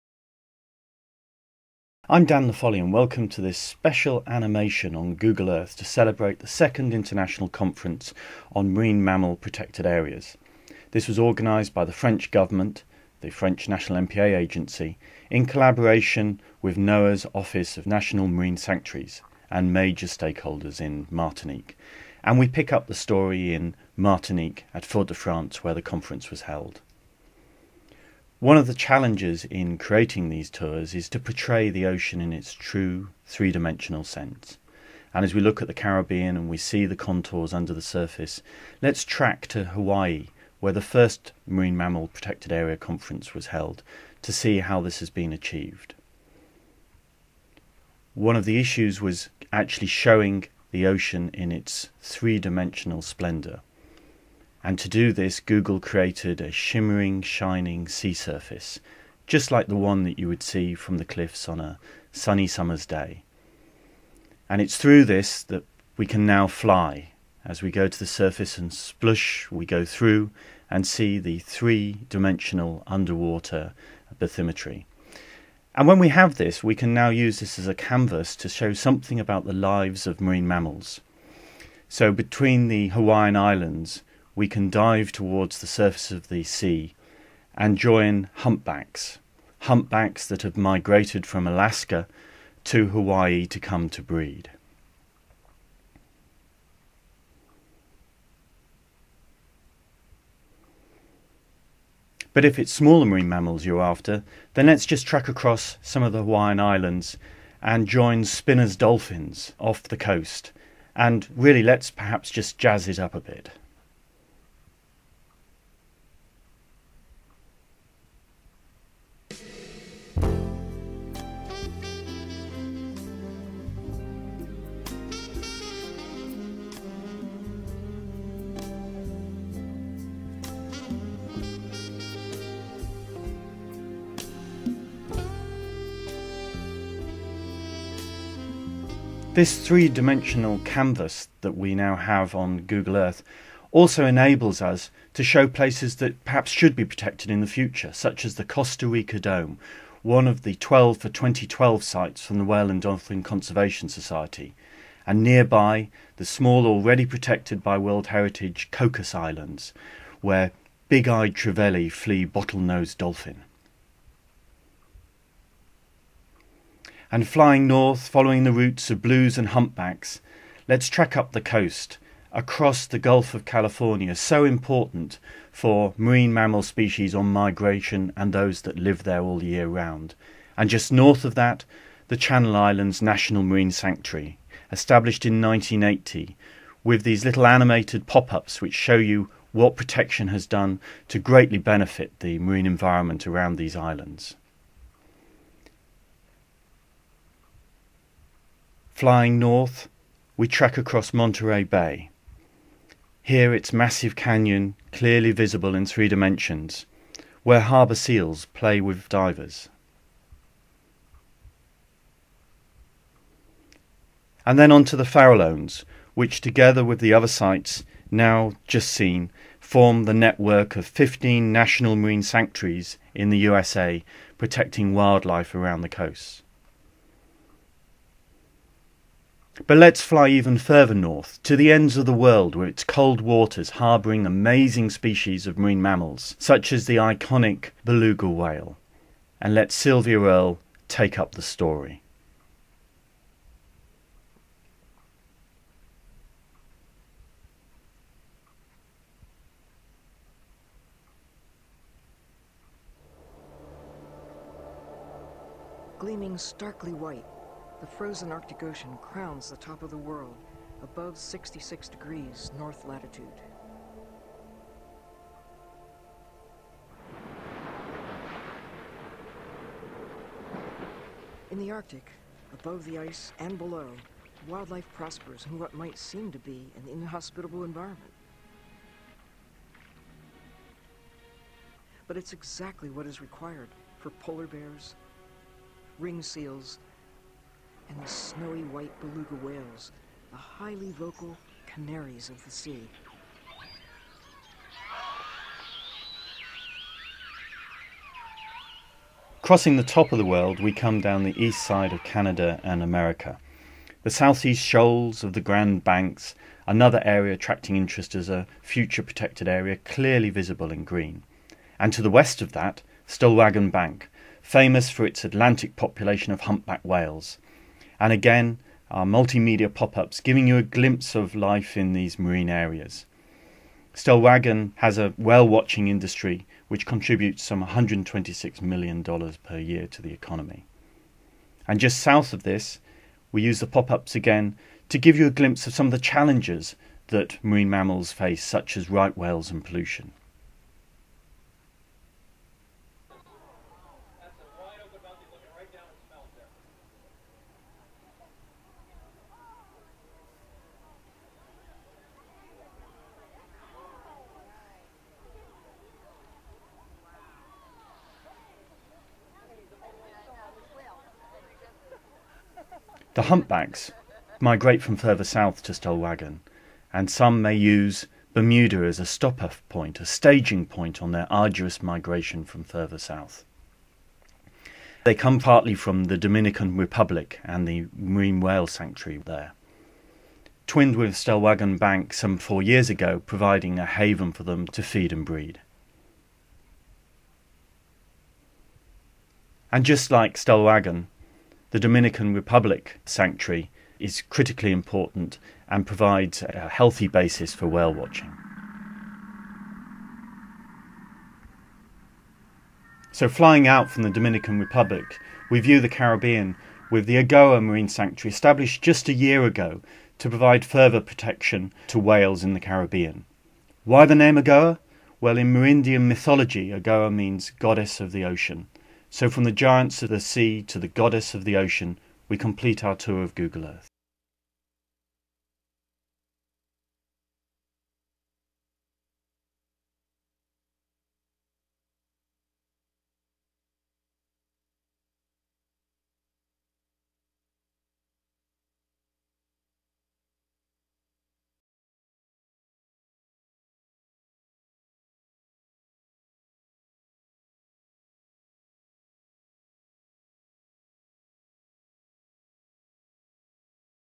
Whale Protected Areas Google Earth Tour
Meet sperm whales, humpback whales and blue whales in the special places where they live. This Google Earth Tour, narrated by Dan Laffoley, Marine Vice Chair of the International Union for Conservation of Nature (IUCN)'s World Commission on Protected Areas, follows the migrations of whales above and below the surface and visits some of the key marine protected areas and sanctuaries for whales around the world. Join the blue whale in the US national marine sanctuaries off California and journey to the proposed Costa Rica Dome Marine Protected Area in the Eastern Tropical Pacific, one of the key areas being proposed as part of the "Homes for Whales" project of the Whale and Dolphin Conservation Society. Follow the humpbacks on migration, from their breeding grounds to the feeding areas in the North Atlantic around the Stellwagen Bank National Marine Sanctuary, and visit one of the most recent protected areas, the Agoa Sanctuary in the French Caribbean. This animated Google Earth Tour was first presented as part of a keynote lecture by Dan Laffoley (IUCN) at the opening of the 2nd International Conference on Marine Mammal Protected Areas (ICMMPA) on 7 November 2011 in the Atrium, Fort-de-France, Martinique. It was prepared with the help of Jenifer Austin Foulkes (Google), Charlotte Vick (Sylvia Earle Alliance/Mission Blue) and Erich Hoyt (Whale and Dolphin Conservation Society/IUCN).